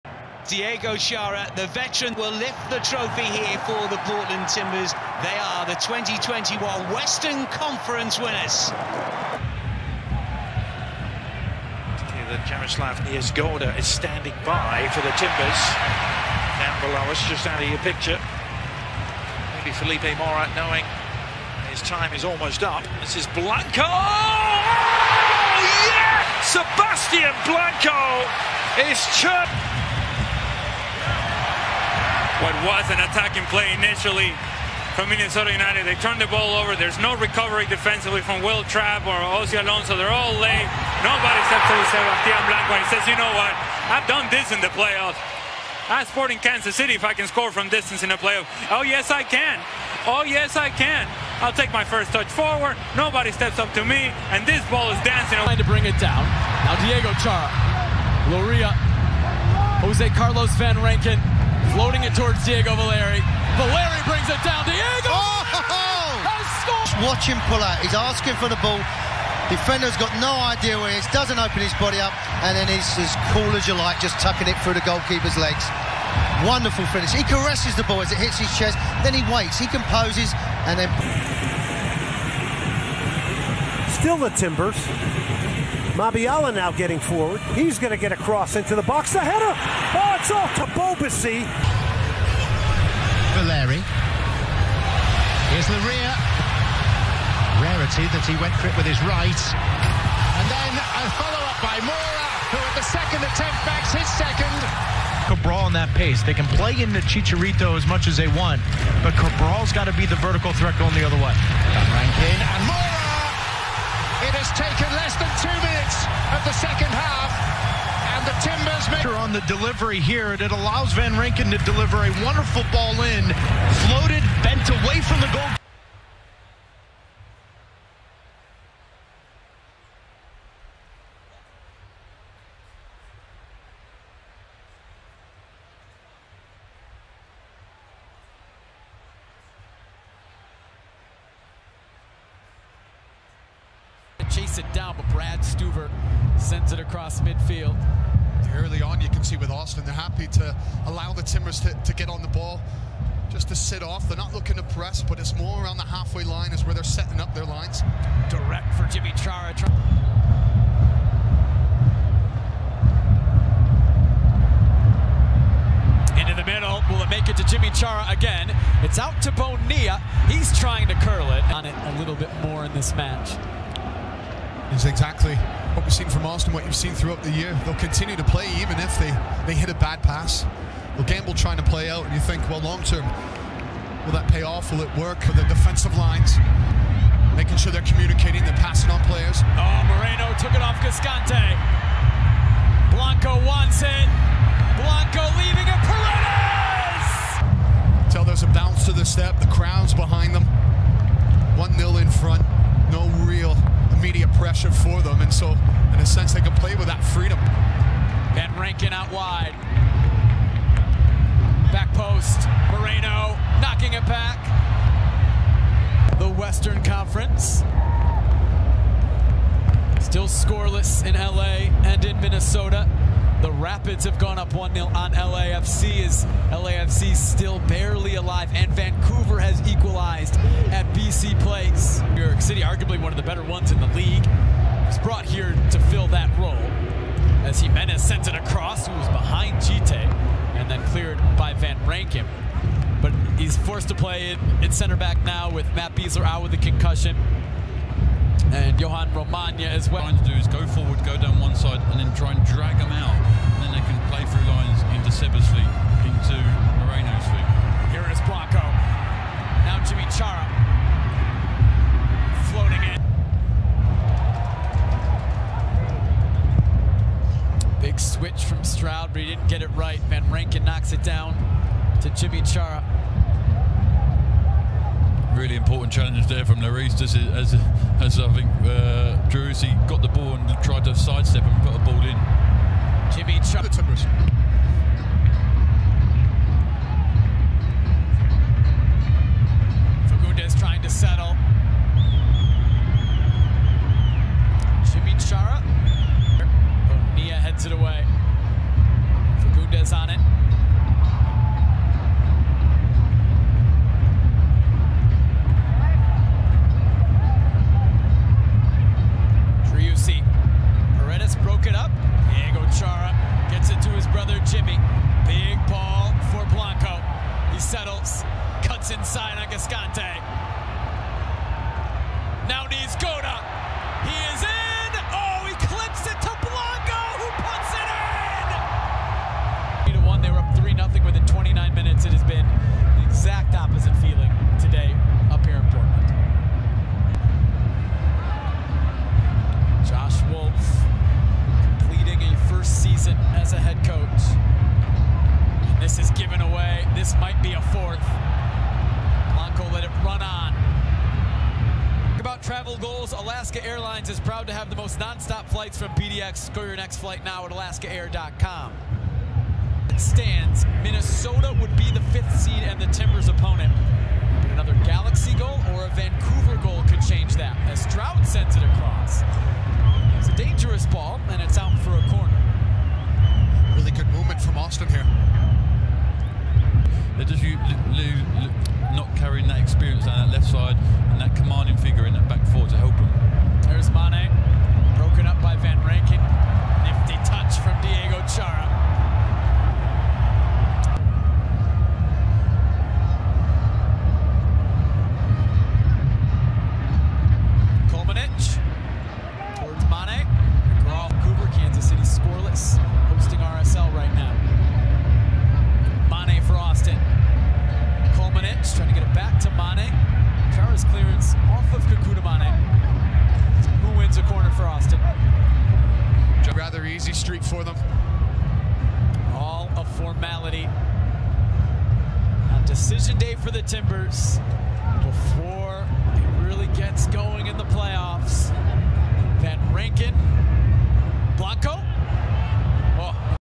Jose Carlos Van Rankin Assists & Defense 2021
Josecarlos Van Rankin Galland defensive skills and 4 assists with Portland Timbers in MLS 2021.

2021